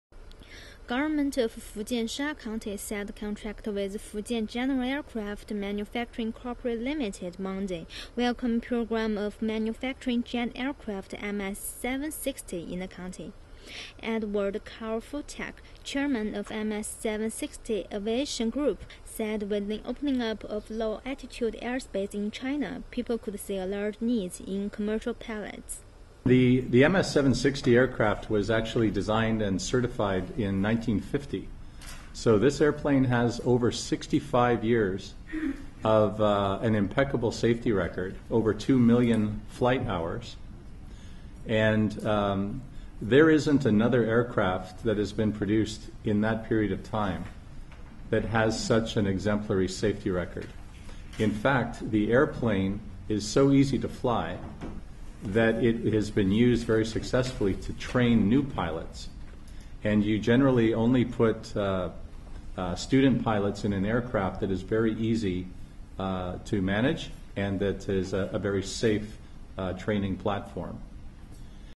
MS760 Commercial jet aircraft manufacturing program in Fujian
Government of Fujian Sha county signed contract with Fujian General Aircraft Manufacturing Co. Ltd. in HK Monday, welcoming program of manufacturing jet aircraft MS760 in the county.

Edward Karl Furtak, chairman of MS760 Aviation Group, said as with the opening up of  low-altitude airspace in China, people could see a large needs in commercial pilots.